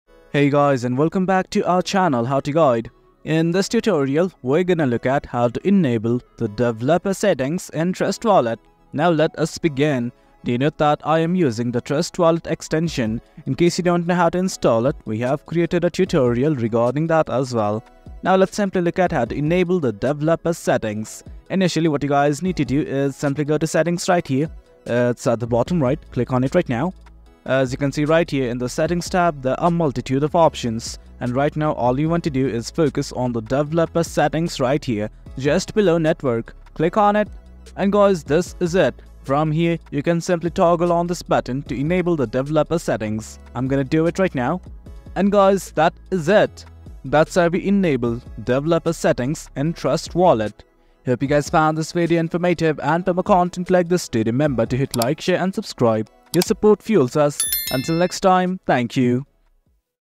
How to Enable Developer Settings on Trust Wallet (2024)
In this video, I will show you how to enable developer settings in Trust Wallet. Developer settings can give you access to advanced features and tools to help manage your cryptocurrencies better. I will guide you step by step through the process, making it easy to follow along.